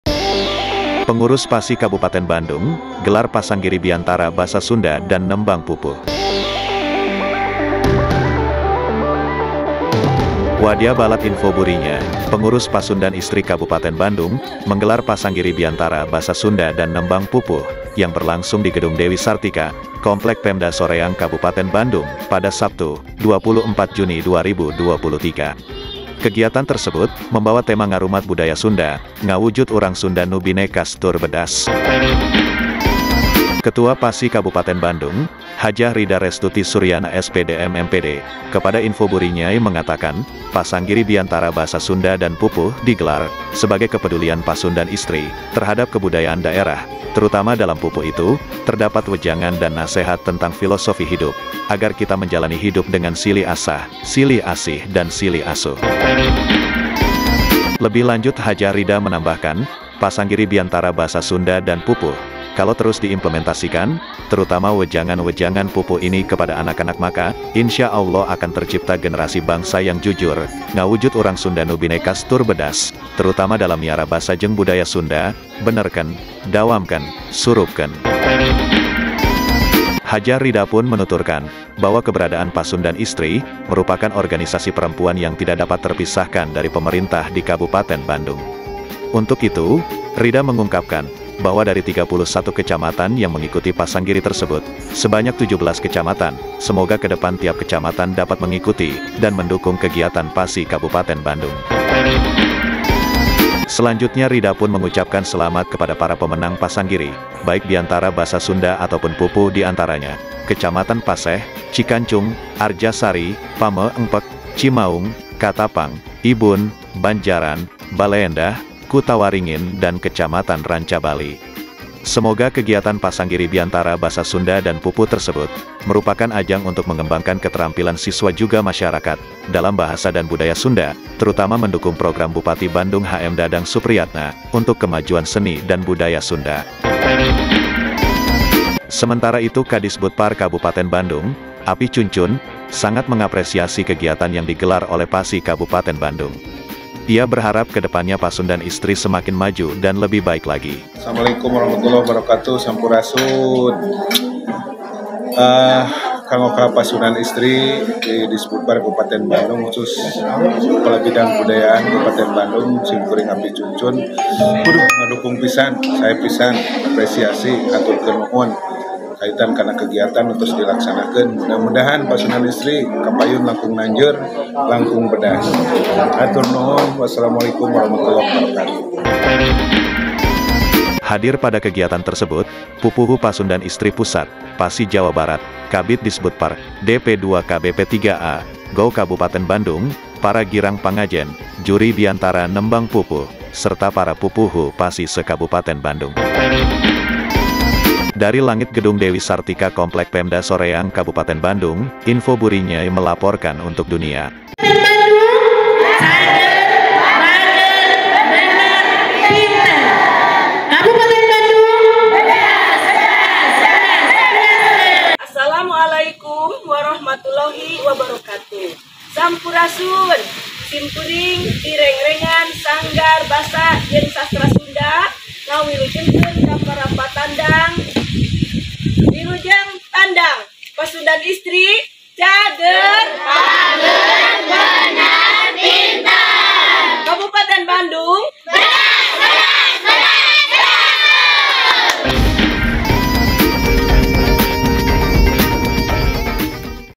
Pengurus Pasi Kabupaten Bandung, Gelar Pasanggiri Biantara Basa Sunda dan Nembang Pupuh
SOREANG, INFO BURINYAY - Pengurus Pasundan Istri Kabupaten Bandung, menggelar Pasanggiri Biantara Basa Sunda dan Nembang Pupuh, yang berlangsung di Gedung Dewi Sartika, Komplek Pemda Soreang Kabupaten Bandung, Sabtu, 24 Juni 2023.

Kegiatan tersebut, membawa tema Ngarumat Budaya Sunda, Ngawujud urang SUnda nu Binekas tur Bedas.

Ketua Pasi Kabupaten Bandung, Hj. Rida Restuti Suryana, S.Pd.,M.MPd, kepada info Burinyai mengatakan,Pasanggiri Biantara Basa Sunda dan Pupuh digelar, sebagai kepedulian Pasundan Istri, terhadap kebudayaan daerah, terutama dalam pupuh itu, terdapat weujangan dan nasehat tentang filosofi hidup, agar kita menjalani hidup dengan silih asah, silih asih dan silih asuh.

Lebih lanjut Hj. Rida menambahkan, Pasanggiri Biantara Basa Sunda dan Pupuh, kalau terus di implementasikan, terutama wejangan wejangan pupuh ini kepada anak-anak maka, Insya Allah akan tercipta generasi bangsa yang jujur, ngawujud urang sunda nu binekas tur Bedas, terutama dalam Miara Basa jeung Budaya Sunda, Benerkeun, Dawamken, Surupkeun.

Hj. Ridapun menuturkan, bahwa keberadaan Pasundan Istri, merupakan organisasi perempuan yang tidak dapat terpisahkan dari pemerintah di Kabupaten Bandung. Untuk itu, Rida mengungkapkan,
bahwa dari 31 kecamatan yang mengikuti  pasanggiri tersebut, sebanyak 17 kecamatan, semoga kedepan  tiap kecamatan dapat mengikuti, dan mendukung kegiatan Pasi Kabupaten Bandung.

Selanjutnya Ridapun mengucapkan selamat kepada para pemenang Pasanggiri,  Baik biantara Basa sunda Ataupun Pupuh di antaranya, Kecamatan Paseh, Cikancung, Arjasari, Pameu ngpeuk, Cimaung, Katapang, ibun, Banjaran, Baleendah, Kutawaringin dan Kecamatan Rancabali.

Semoga Kegiatan pasanggiri Biantara Basa Sunda dan Pupuh tersebut, merupakan ajang untuk mengembangkan keterampilan siswa juga Masyarakat, dalam bahasa dan budaya Sunda, terutama
Mendukung Program Bupati Bandung H.M. Dadang Supriatna, untuk kemajuan seni dan Budaya Sunda.

Sementara itu Kadisbudpar Kabupaten Bandung, Apih Cuncun, sangat mengapresiasi Kegiatan yang di gelar oleh Pasi Kabupaten Bandung. Ia  berharap Kedepannya Pasundan Istri semakin maju dan lebih baik lagi.

Hadir Pada Kegiatan tersebut, Pupuhu Pasundan Istri Pusat, PASI  Jawa Barat, KABID DISBUDPAR, DP2KB P3A,GOW Kabupaten Bandung , Para Girang Pangajen, Juri Biantara Nembang Pupuh, serta Para Pupuhu Pasi Se Kabupaten Bandung.

Liputan Team Info Burinyay Dari Gedung Dewi Sartika Komplek Pemda Soreang Kabupaten Bandung.